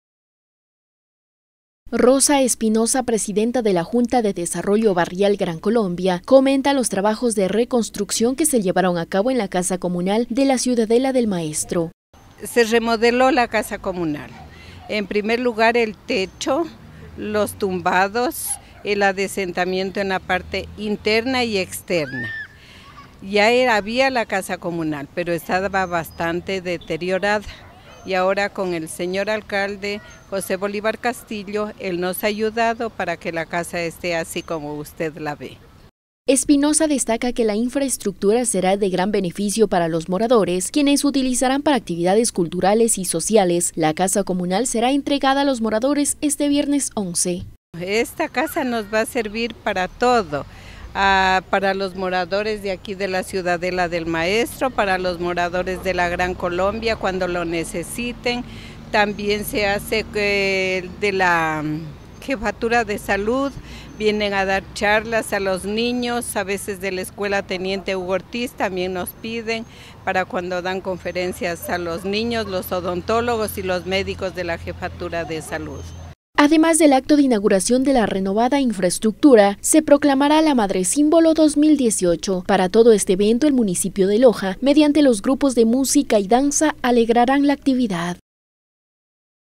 Ciudadela El Maestro recibirá casa comunal remodelada
En la ciudadela El Maestro de la ciudad de Loja, será inaugurada una nueva obra el próximo viernes 11 de mayo,  a las 20h00.

La obra consiste en la remodelación de la casa comunal que estaba deteriorada en su totalidad, por ello fue necesario intervenir en el techo, tumbados, adecentamiento interno y externo, pisos, puerta principal. Este proyecto se hace realidad gracias al aporte del Municipio de Loja y la organización de la Junta Barrial.